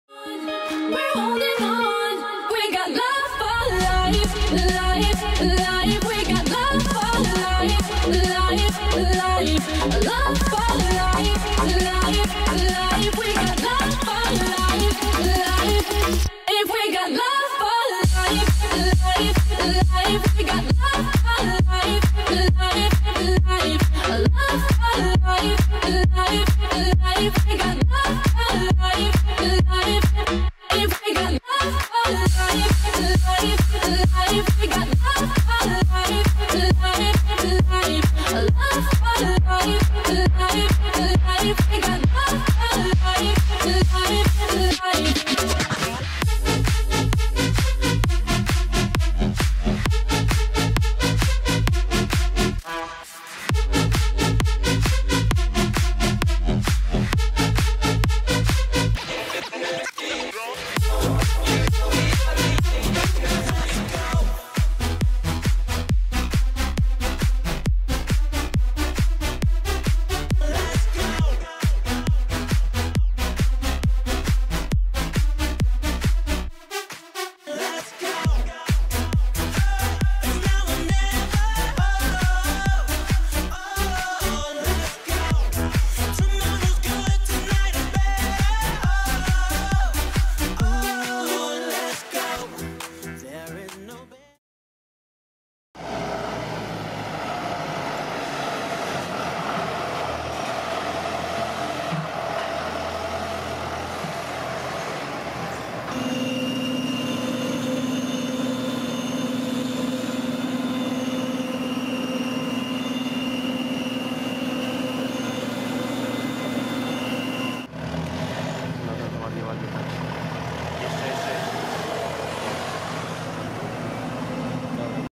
*MOKRE* Żniwa 2O2O ㋡ 2x John Deere | Mf 255 ㋡ Wielkopolska | Kumcio Polska
1 ha jęczmienia wydajność ok 8 t .

Najlepszy Grafik ✔️